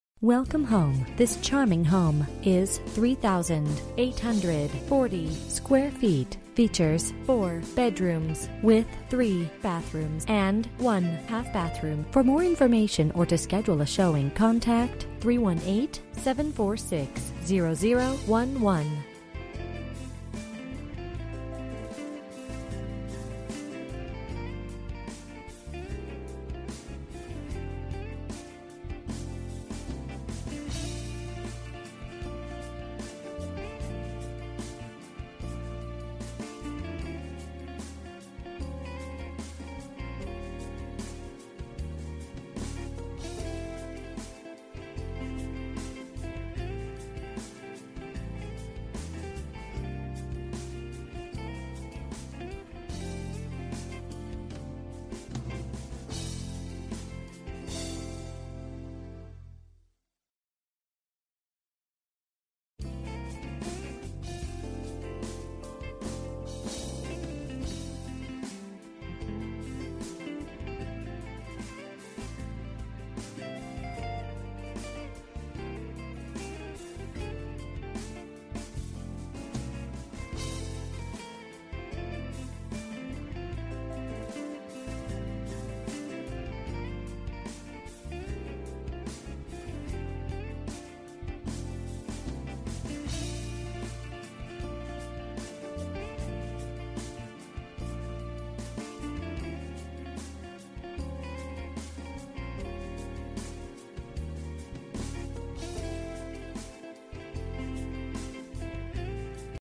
Residential for sale - 116 Old Bellevue Road, Benton, LA 71006
Great Unique House! New Architectural Roof~ Fresh Paint~1000 Sq.Ft. Guest Home Attached To Main Home Including Full Kitchen, Den & Dining Room~ Gorgeous Inground Pool Perfect For Entertaining! Up To 7 Acres Available For Sale~ Fenced For Horses~ This Is Such A Unique Property And Would Be Perfect For Two Families Or Just One Family That Likes To Entertain But Doesn't Want Anyone To See Your Personal Kitchen ;))

Bedrooms: 4
Bathrooms: 3 full and 1 half Bathrooms
Square Feet: 3840
Price: $375,000

05/12/2017 06:46:03 pm

Last modified: